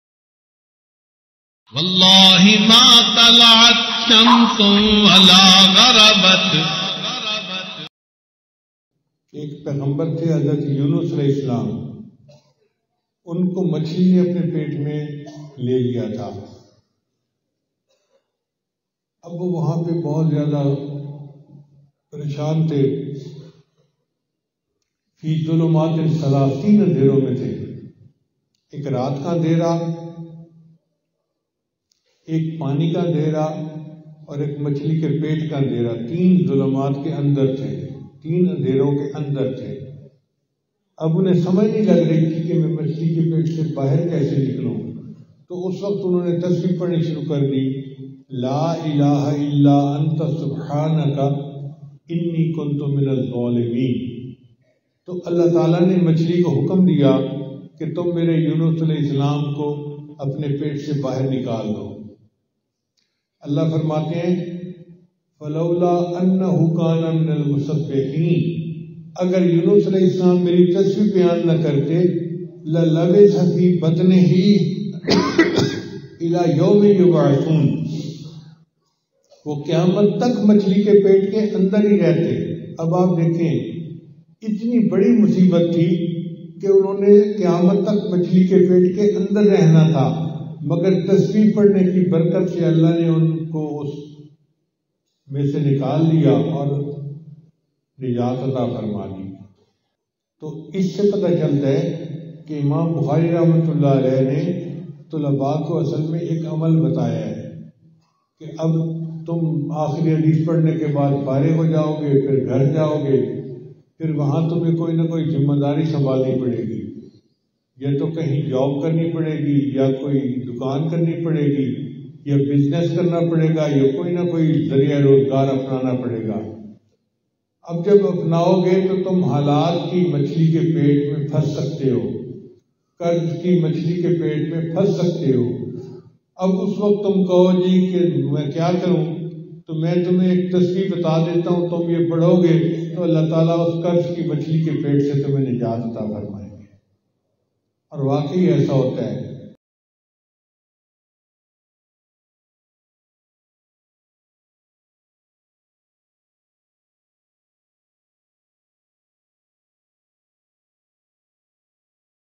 Rizq Ki Tangi Khatam Karnay Ka Asan Aur Chota Amal - رزق کی تنگی ختم کرنے کا چھوٹا اور آسان عمل
Rizq Ki Tangi Khatam Karnay Ka Asan Aur Chota Amal - Peer Zulfiqar Ahmad Naqshbandi DBA - رزق کی تنگی ختم کرنے کا چھوٹا اور آسان عمل

🖥️ OFFICIAL ACCOUNTS✔️